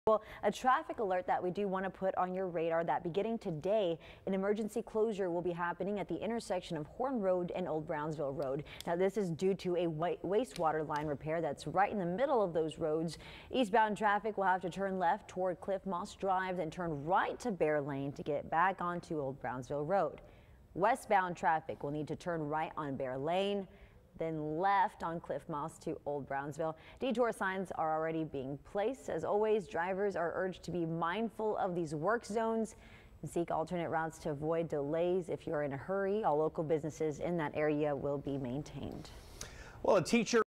Emergency road closure near Gabe Lozano golf course starting Friday
Detour signs were already being placed at the intersection of Horne Road and Old Brownsville Road on Friday evening.